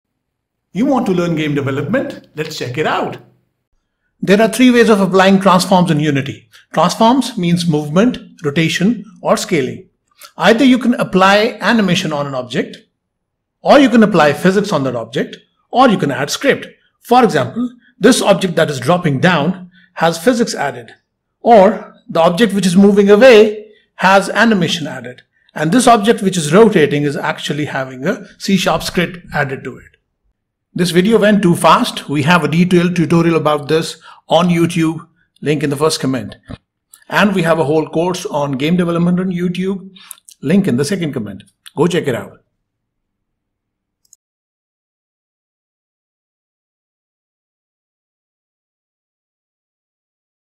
Transforms: 3 ways of moving, rotating and resizing objects in Unity in a game scene [English]
Three ways of applying transforms in Unity during game play

When a player is playing the game, he/she may be controlling some game objects but there are so many other game objects in the scene that change as per the game scenario. The three transforms that take effect on different objects are:

Translate (movement)
Rotate (rotation)
Scale (resizing)

These three transforms can be applied to any objects in the game based on the game scenario. This video describes the three ways of applying these transforms either on a game even bases or automatically on continuous bases.

You can apply these transforms by:

* Applying Game Physics
* Adding a script
* Adding Animation